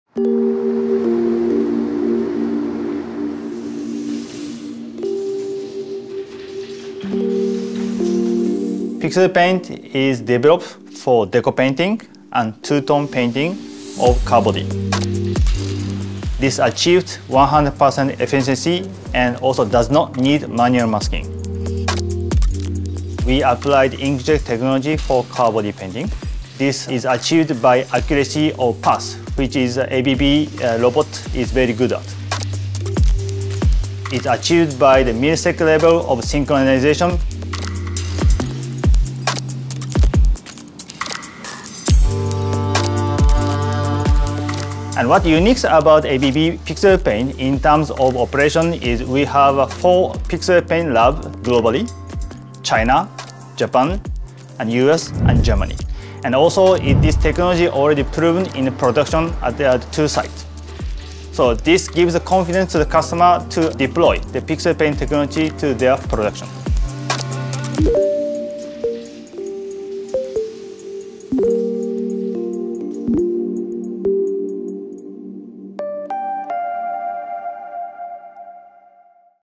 PixelPaint - Inkjet technology for car body painting
ABB’s PixelPaint non-overspray technology eliminates the need for time-consuming masking and demasking methods for two-tone painting in the automotive industry, significantly reducing time and costs, improving productivity, and expanding possibilities for customized paint schemes to meet customer demand.